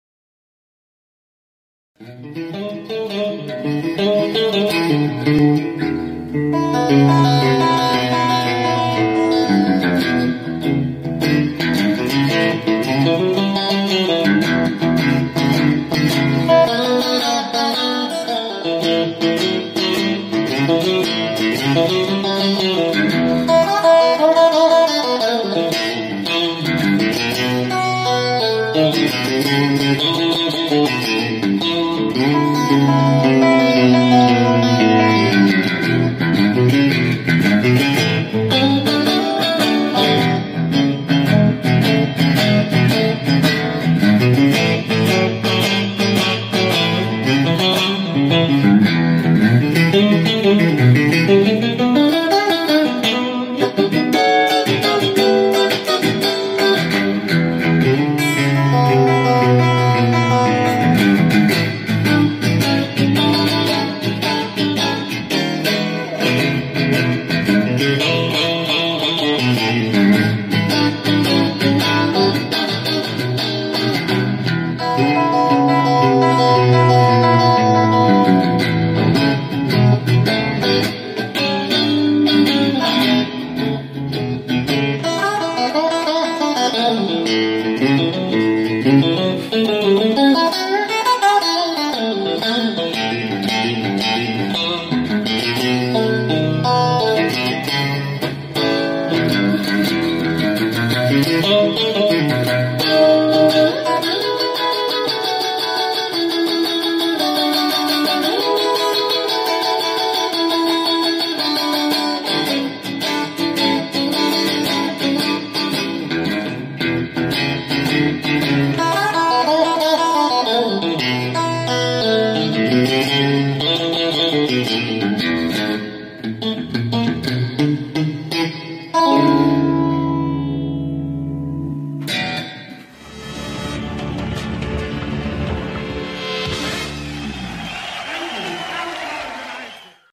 Blues Shuffle impro Pablo Carvalho en El Tremolo
Esta es una impro de blues shuffle que forma parte de la nota que hice para el canal de guitarra El Tremolo.
La entrevista completa la podes ver aca⬇️⬇️⬇️⬇️